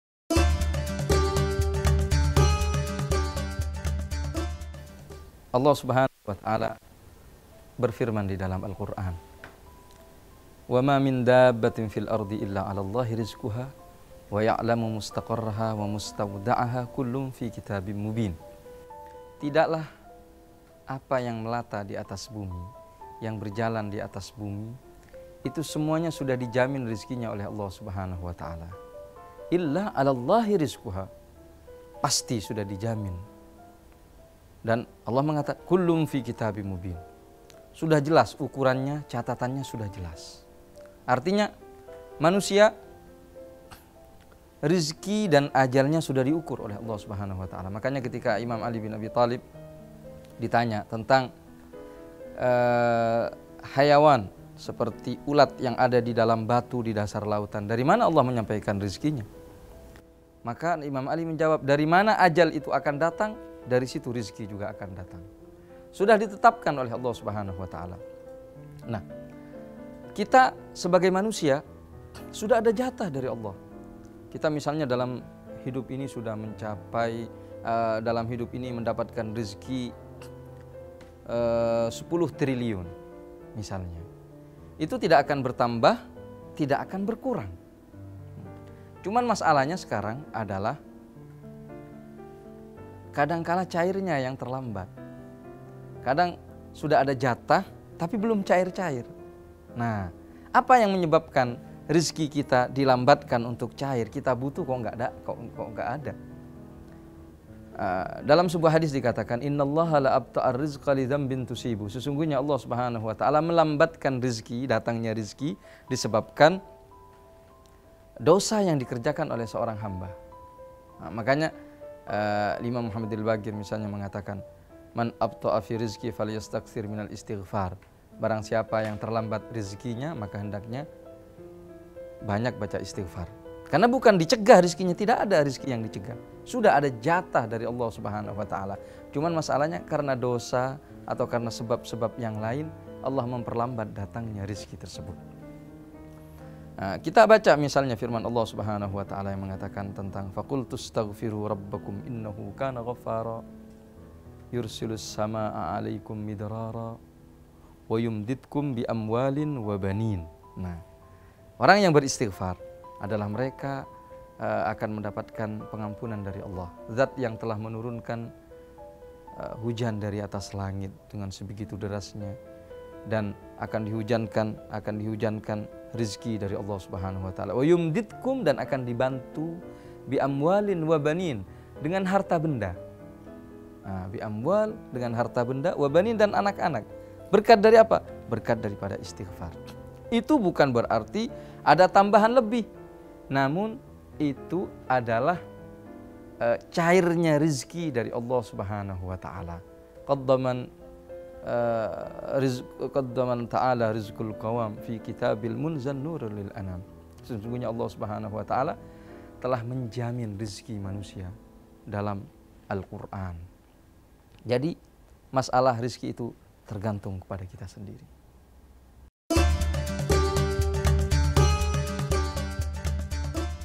Hidup Ini Indah #2 Semua Sudah Dijamin Rezekinya - Habib Hamid Al Qadry
Hidup Ini Indah #2
Semua Sudah Dijamin Rezekinya -
Habib Hamid Al Qadry

Download aplikasi Nabawi TV Official untuk kemudahan dalam menyaksikan tayangan Nabawi TV di:
Google Playstore: